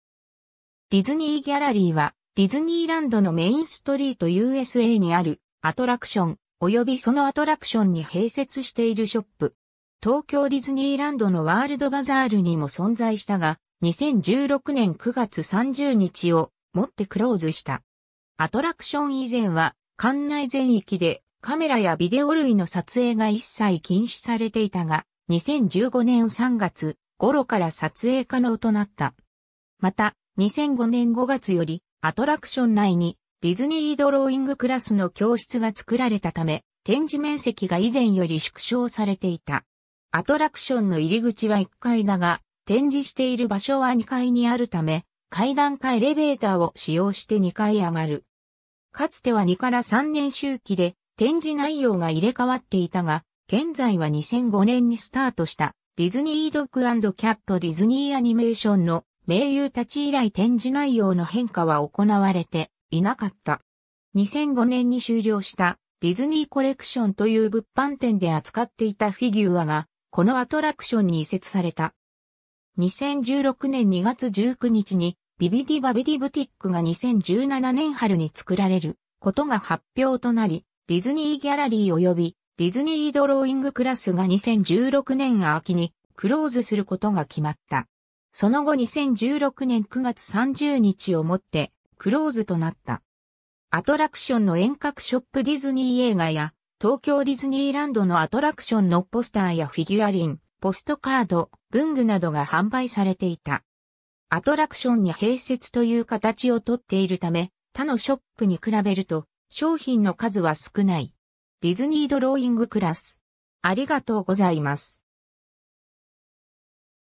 ディズニーギャラリー
ディズニーギャラリー（The Disney Gallery）は、ディズニーランドのメインストリートUSAにある、アトラクション、およびそのアトラクションに併設しているショップ。
東京ディズニーランドのワールドバザールにも存在したが、2016年9月30日をもってクローズした。
 アトラクション 以前は館内全域でカメラやビデオ類の撮影が一切禁止されていたが、2015年3月ごろから撮影可能となった。
また、2005年5月よりアトラクション内に「ディズニードローイングクラス」の教室が作られたため、展示面積が以前より縮小されていた。
 アトラクションの入り口は1階だが、展示している場所は2階にあるため、階段かエレベーターを使用して2階へ上がる。
 かつては2-3年周期で展示内容が入れ替わっていたが、現在は2005年にスタートした『「ディズニー・ドッグ＆キャット」〜ディズニーアニメーションの名優たち〜』以来展示内容の変化は行われていなかった。
 2005年に終了した「ディズニーコレクション」という物販店で扱っていたフィギュア（人形）が、このアトラクションに移設された。
 2016年2月19日にビビディ・バビディ・ブティックが2017年春に作られることが発表となり、「ディズニーギャラリー」および「ディズニードローイングクラス」が2016年秋にクローズすることが決まった。
その後2016年9月30日をもってクローズとなった。
 アトラクションの沿革 ショップ ディズニー映画や東京ディズニーランドのアトラクションのポスターやフィギュアリン、ポストカード、文具などが販売されていた。
アトラクションに併設という形をとっているため、他のショップに比べると商品の数は少ない。
 ディズニードローイングクラス